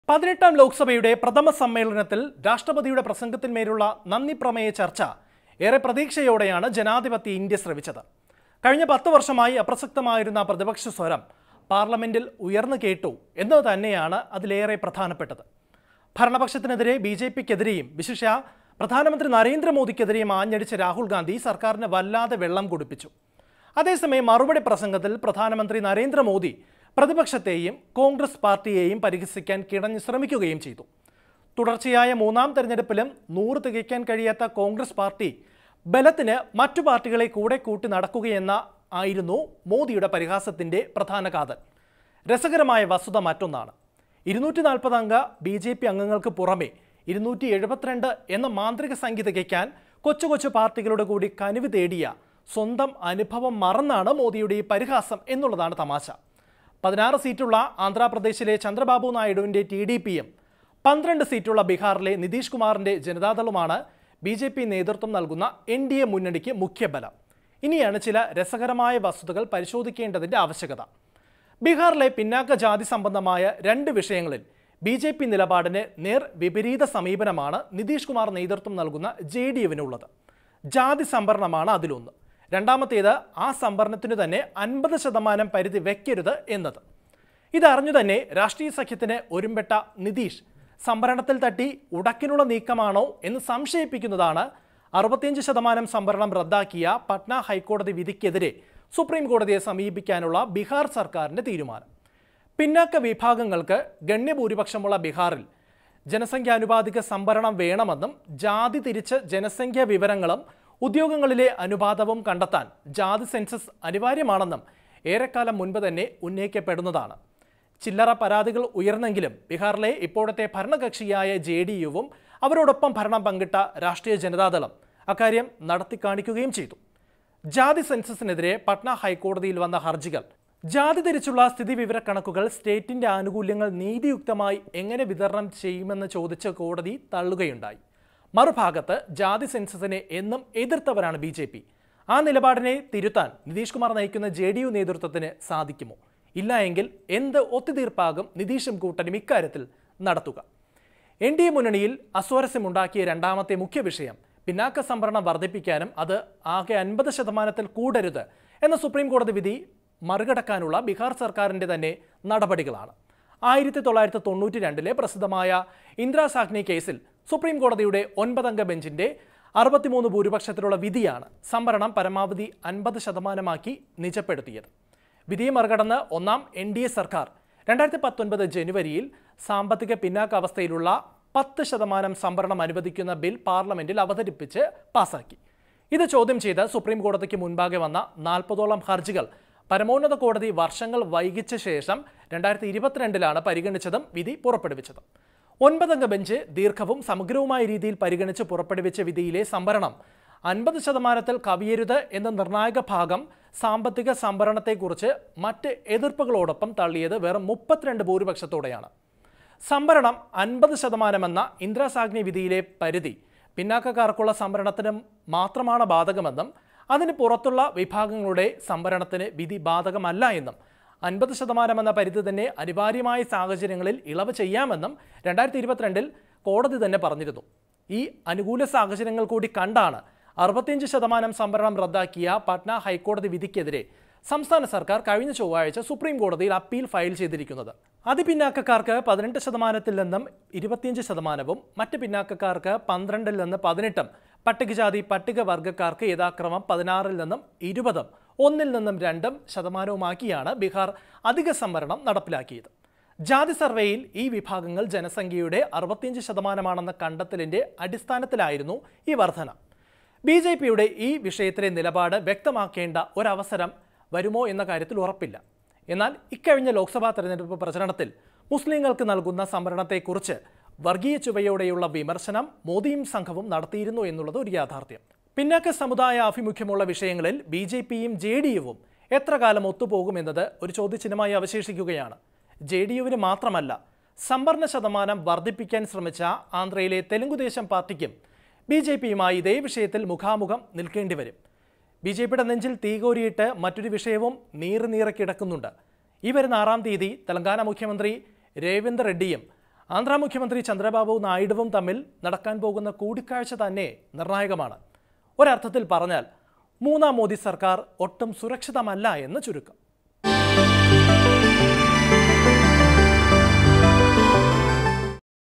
സംവരണത്തിൽ തപ്പിത്തടഞ്ഞ് മോദി- നിതീഷ് കൂട്ടുകെട്ട്; എൻഡിഎ സർക്കാർ വീഴുമോ? | PM Modi | Nitish Kumar
പിന്നാക്ക സമുദായാഭിമുഖ്യമുള്ള വിഷയങ്ങളിൽ ബി.ജെ.പിയും ജെ.ഡി.യുവും എത്രകാലം ഒത്തുപോകുമെന്നത് ഒരു ചോദ്യചിഹ്നമാണ്. ജെ.ഡി.യുവിനു മാത്രമല്ല, സംവരണ ശതമാനം വർധിപ്പിക്കാൻ ശ്രമിച്ച ആന്ധ്രയിലെ തെലുഗദേശം പാർട്ടിക്കും ബി.ജെ.പിയുമായി ഇതേ വിഷയത്തിൽ മുഖാമുഖം നിൽക്കേണ്ടിവരും. ബിജെപിയുടെ നെഞ്ചിൽ തീ കോരിയിട്ട് മറ്റൊരു വിഷയവും നീറി നീറി കിടക്കുന്നുണ്ട്. ആറാം തിയതി തെലങ്കാന മുഖ്യമന്ത്രി രേവന്ത് റെഡ്ഡിയും, ആന്ധ്രാമുഖ്യമന്ത്രി ചന്ദ്രബാബു നായ്ഡുവും തമ്മിൽ നടക്കാൻ പോകുന്ന കൂടിക്കാഴ്ച നിർണായകമാണ്. ഒരർഥത്തിൽ പറഞ്ഞാൽ മൂന്നാം മോദി സർക്കാർ ഒട്ടും സുരക്ഷിതമല്ല എന്ന് ചുരുക്കം...